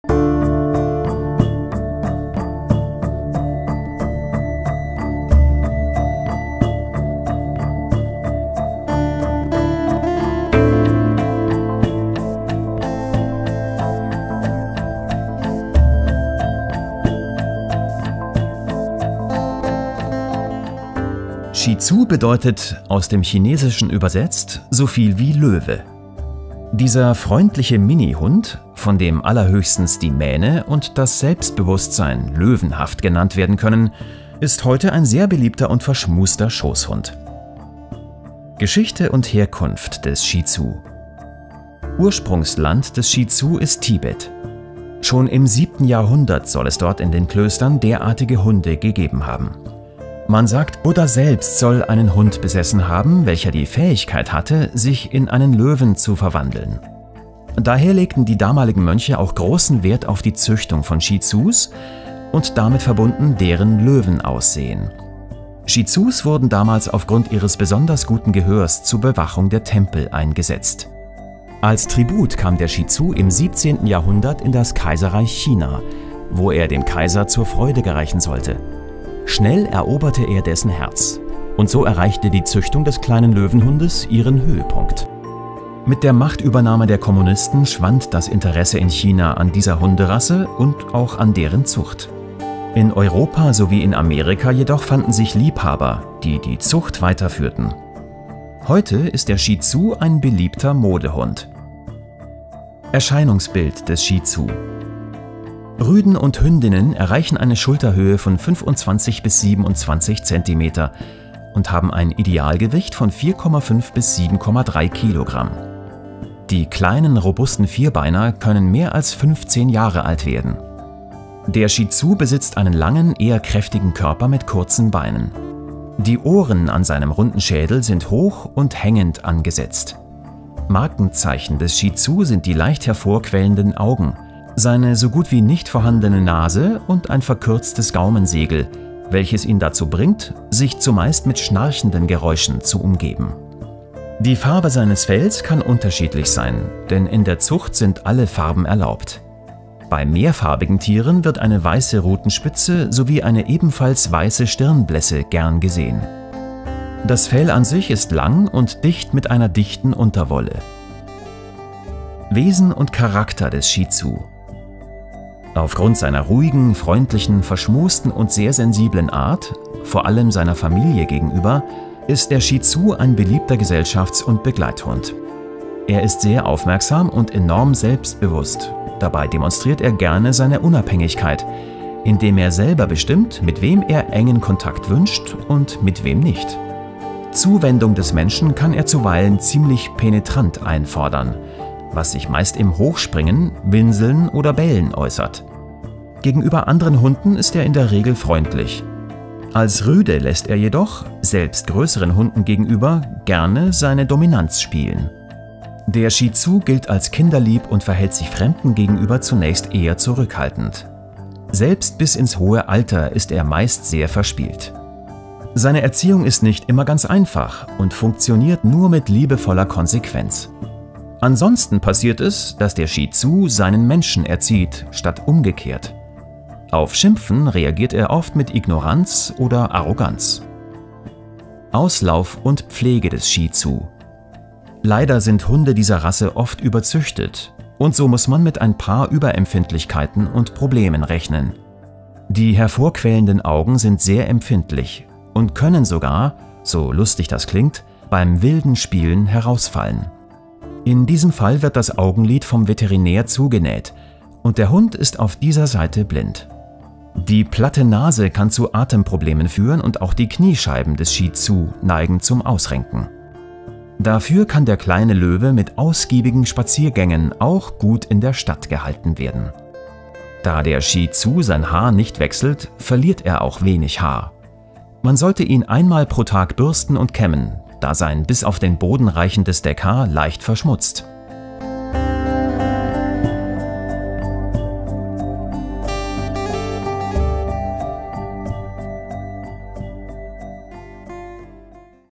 ► Shih Tzu [2020] Rasse, Aussehen & Charakter
In dieser Rassebeschreibung findet ihr alle wichtigen Infos zum Shih Tzu:
► Intro 0:00
► Shih Tzu Geschichte & Herkunft 0:38
► Shih Tzu Erscheinungsbild & Aussehen 1:42
► Shih Tzu Wesen & Charakter 2:41
► Shih Tzu Auslauf, Pflege & Krankheiten 3:51

Shih Tzu Rasseportrait [2020] Informativer Praxisratgeber
Cooles Shih Tzu T-Shirt
Süße Shih Tzu Kaffeetasse